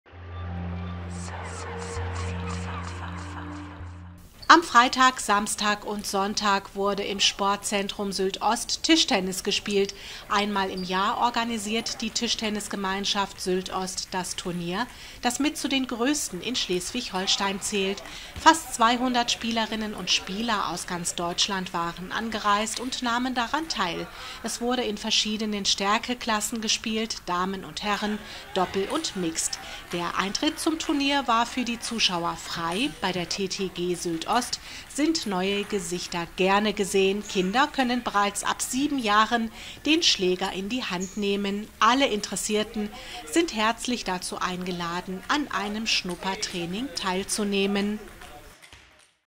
Tischtennismeisterschaft auf Sylt
Die Tischtennisgemeinschaft Sylt Ost veranstaltete die offene Sylter Meisterschaft.